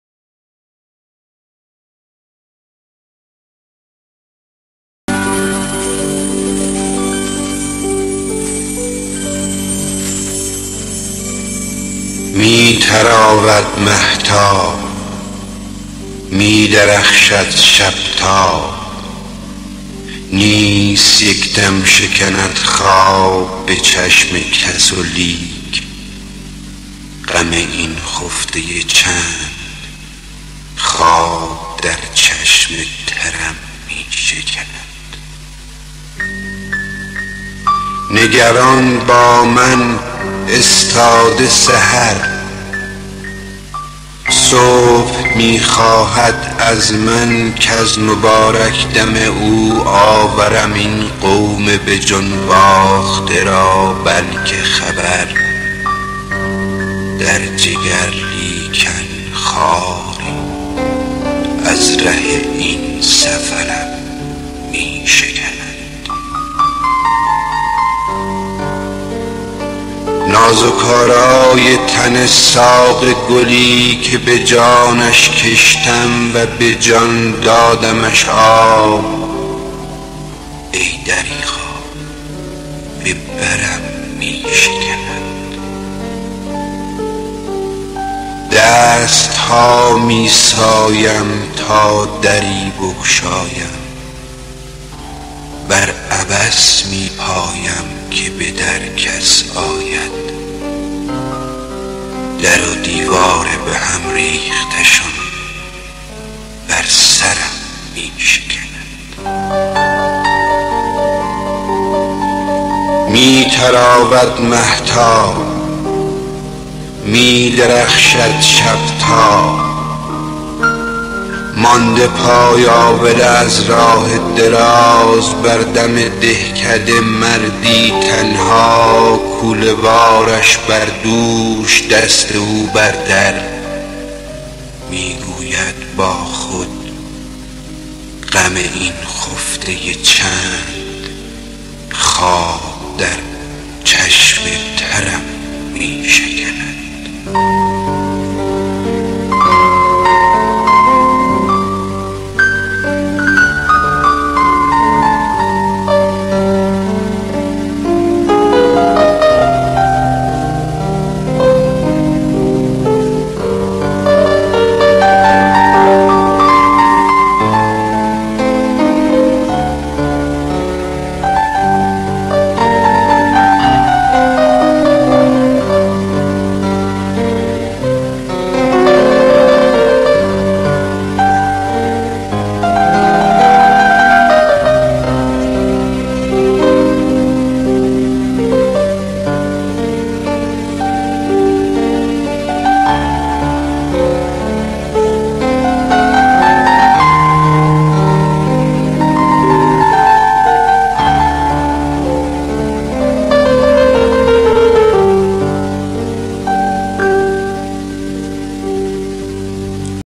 نیما یوشیج - مهتاب Nima Yushij
نیما یوشیج
شعر:مهتاب
دکلمه:اسماعیل جنتی
آهنگ:بهرام دهقانیار